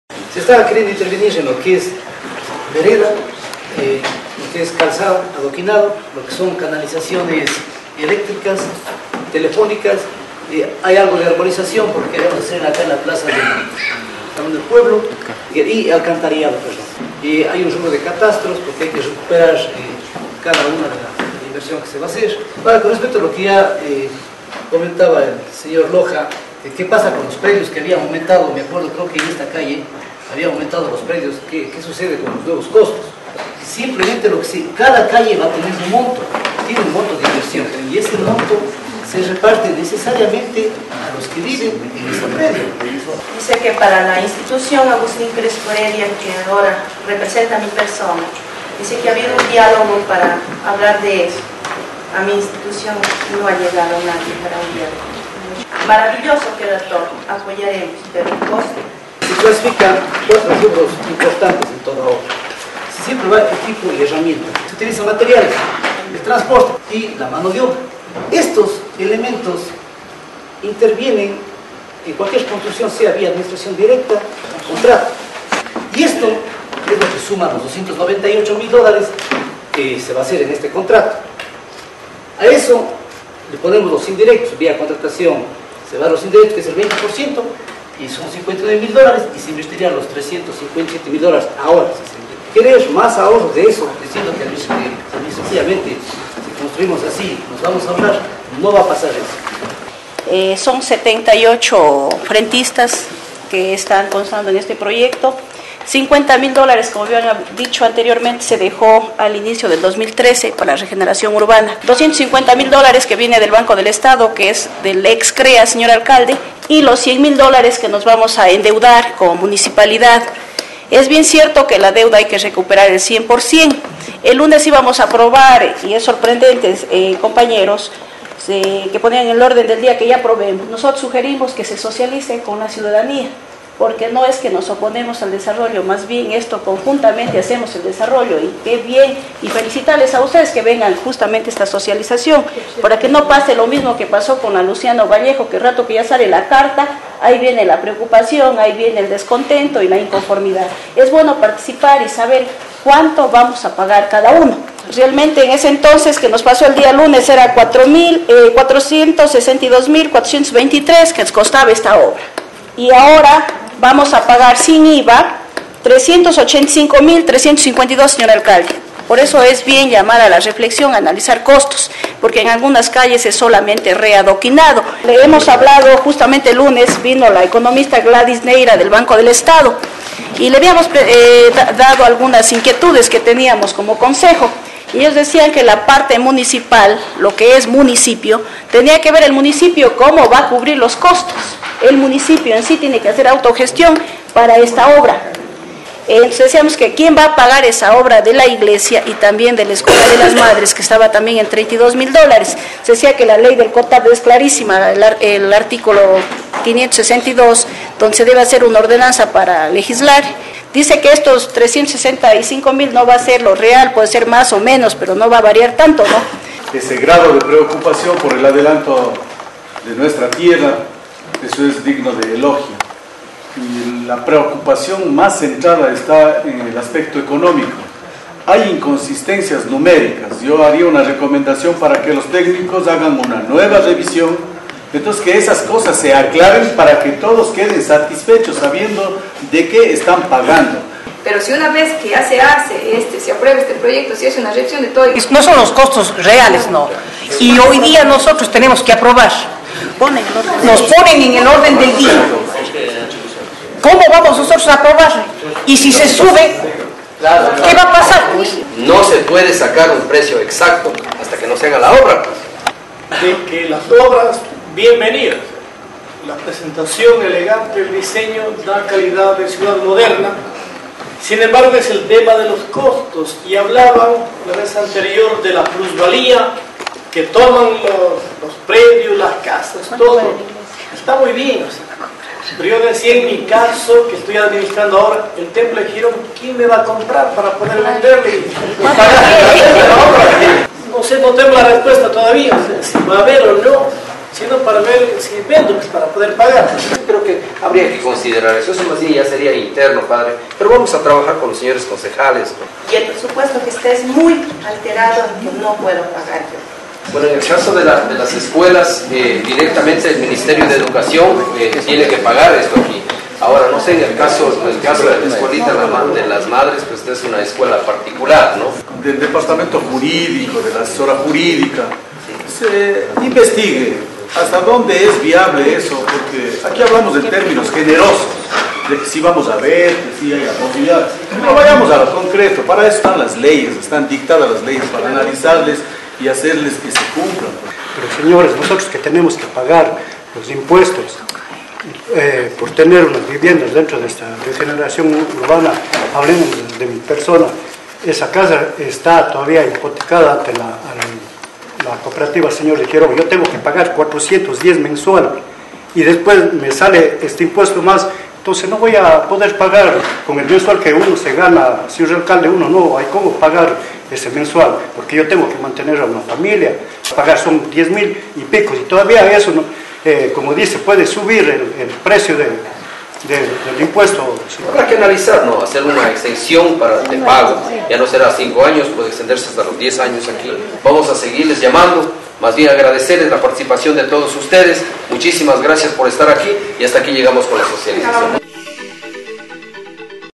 Costos de regeneración bajaron
Luego de la presentación del proyecto se dio a conocer el costo de las obras a ejecutarse, mismos que han bajado considerablemente, se estipula en 80,000 dólares la diferencia del valor inicial con el presupuesto que ahora se maneja, sin embargo la colectividad no está conforme y tiene dudas. Web